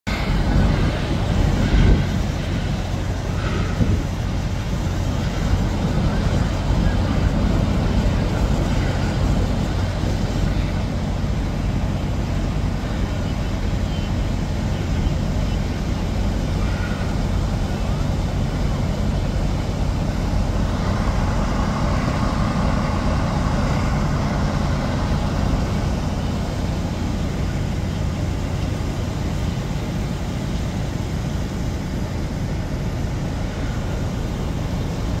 You can see the 3 master streams operating at this 2nd Alarm in Port Chester Rye Brook L30,
Greenwich Truck 1, & West Harrison TL9
12/3/24